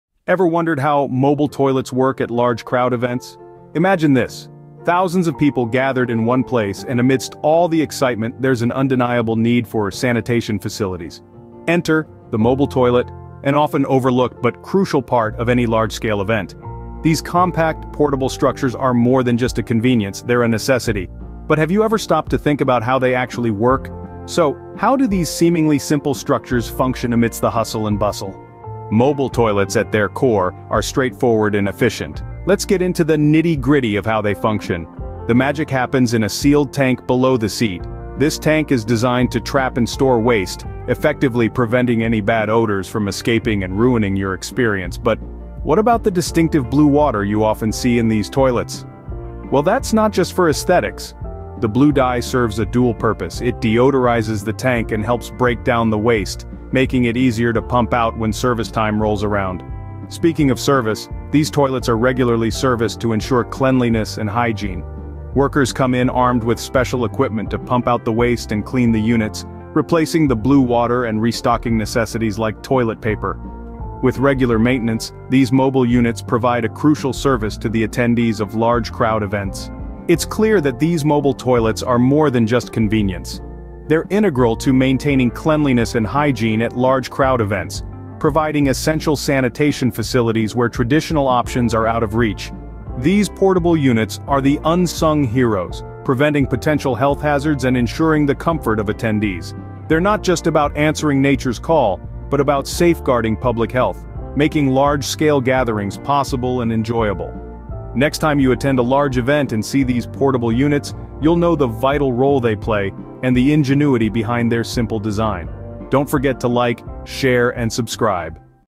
You didn't know that: Mobile Toilets (1080)
Ever wondered how mobile toilets work at large crowd events? Imagine this: thousands of people gathered in one place, and amidst all the excitement, there's an undeniable need for sanitation facilities.
Enter, the mobile toilet, an often overlooked but crucial part of any large-scale event.
  These compact, portable structures are more than just a convenience, they're a necessity.
Mobile toilets, at their core, are straightforward and efficient. Let's get into the nitty-gritty of how they function. The magic happens in a sealed tank below the seat.
  This tank is designed to trap and store waste, effectively preventing any bad odors from escaping and ruining your experience.
  But what about the distinctive blue water you often see in these toilets?
The blue dye serves a dual purpose: it deodorizes the tank and helps break down the waste, making it easier to pump out when service time rolls around.
   Maintenance workers come in armed with special equipment to pump out the waste and clean the units, replacing the blue water and restocking necessities like toilet paper.

timeline
00:00  Intro: The Mystery of Mobile Toilets
00:30 The Inner Workings of Mobile Toilets
01:20 The Importance of Mobile Toilets